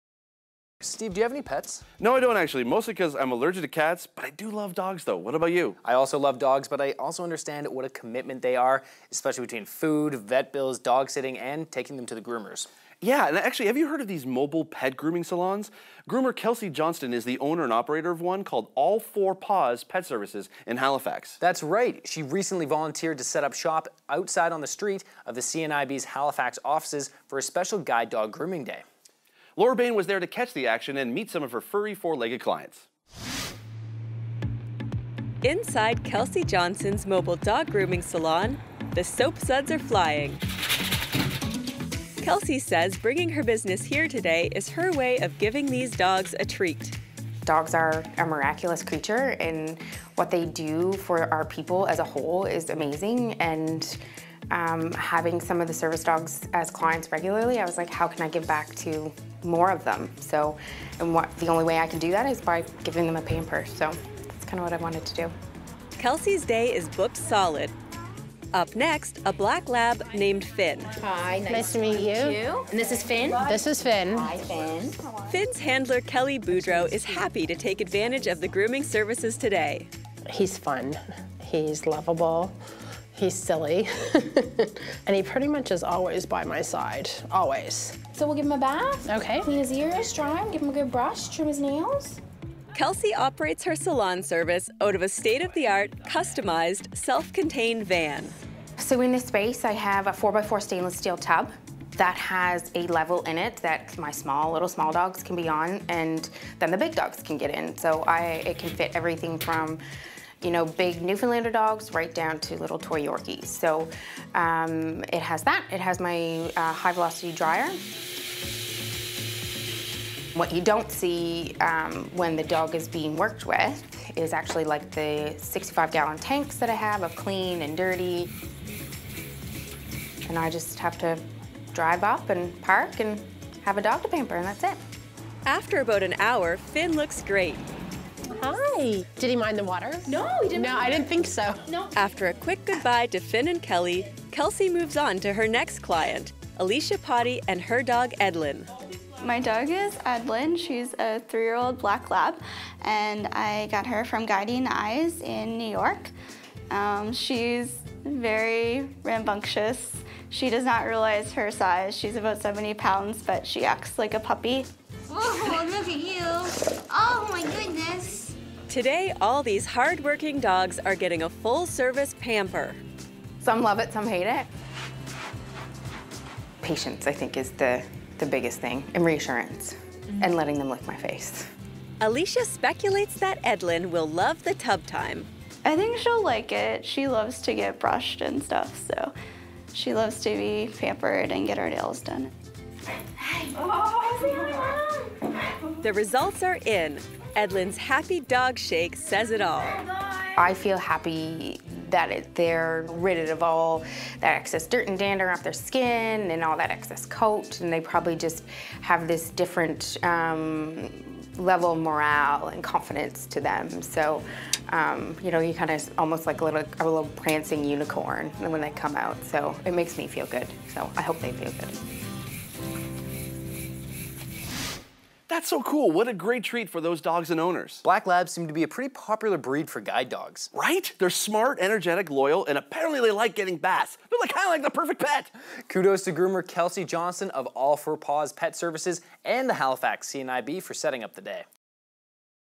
CNIB Dog Wash, Halifax
Kelsey Johnston is the owner of All 4 Paws Pet Services, a unique mobile pet grooming salon that recently teamed up with CNIB Atlantic INCA atlantique for a special Guide Dog "spa day".